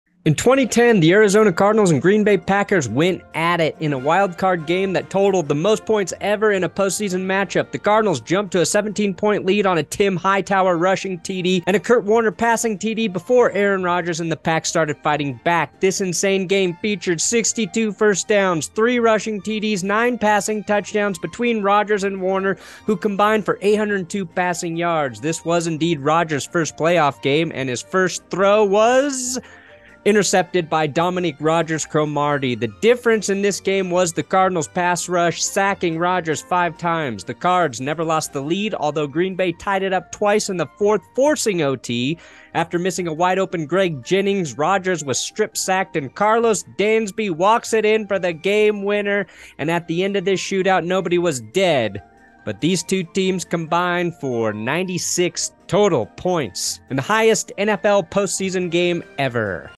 Highest Scoring Playoff Game in NFL History
The Packers and Cardinals went at it in the highest combined score in an NFL playoff game.